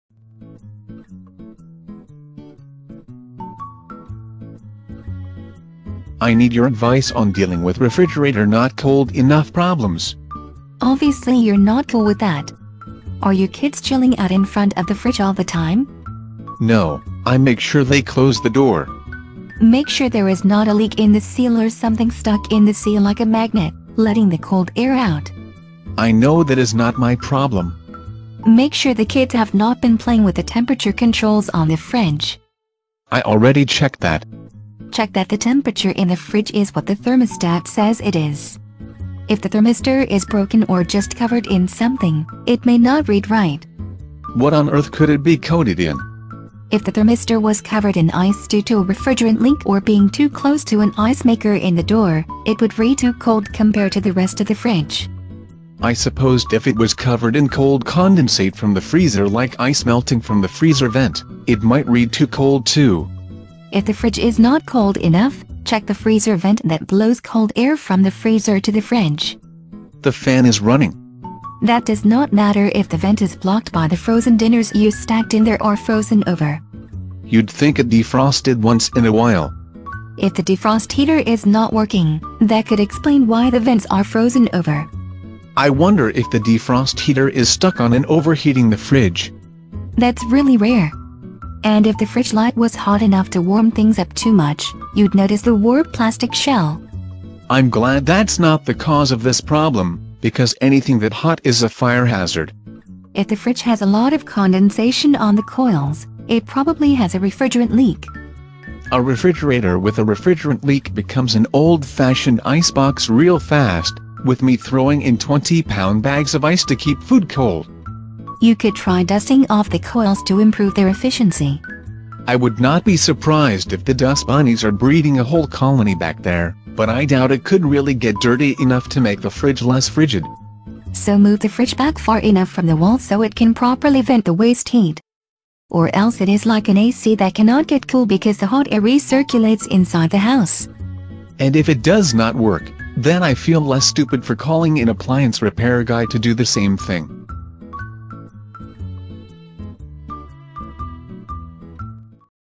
Refrigerator Not Cold Enough Problems
I need your advice on dealing with refrigerator not cold enough problems.

Obviously you’re not cool with that. Are your kids chilling out in front of the fridge all the time?

No, I make sure they close the door.

Make sure there is not a leak in the seal or something stuck in the seal like a magnet, letting the cold air out.

I know that is not my problem.

Make sure the kids have not been playing with the temperature controls on the fridge.

I already checked that.

Check that the temperature in the fridge is what the thermostat says it is. If the thermistor is broken or just covered in something, it may not read right.

What on Earth could it be coated in?

If the thermistor was covered in ice due to a refrigerant leak or being too close to an ice maker in the door, it would read too cold compared to the rest of the fridge.

I supposed if it was covered in cold condensate from the freezer like ice melting from the freezer vent, it might read too cold too.

If the fridge is not cold enough, check the freezer vent that blows cold air from the freezer to the fridge.

The fan is running.

That does not matter if the vent is blocked by the frozen dinners you stacked in there or frozen over.

You’d think it defrosted once in a while.

If the defrost heater is not working, that could explain why the vents are frozen over.

I wonder if the defrost heater is stuck on and overheating the fridge.

That’s really rare. And if the fridge light was hot enough to warm things up too much, you’d notice the warped plastic shell.

I’m glad that’s not the cause of this problem, because anything that hot is a fire hazard.

If the fridge has a lot of condensation on the coils, it probably has a refrigerant leak.

A refrigerator with a refrigerant leak becomes an old fashioned ice box real fast, with me throwing in 20 pound bags of ice to keep food cold.

You could try dusting off the coils to improve their efficiency.

I would not be surprised if the dust bunnies are breeding a whole colony back there, but I doubt it could really get dirty enough to make the fridge less frigid.

So move the fridge back far enough from the wall so it can properly vent the waste heat, or else it is like an AC that cannot get cool because the hot air re-circulates inside the house.

And if it does not work, then I feel less stupid for calling an appliance repair guy to do the same thing.